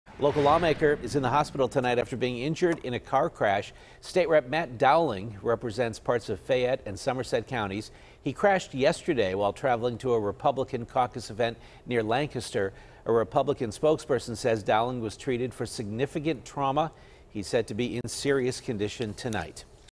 Pa. Lawmaker Hospitalized After Crash
A local state representative is in "serious but stable" condition after a crash.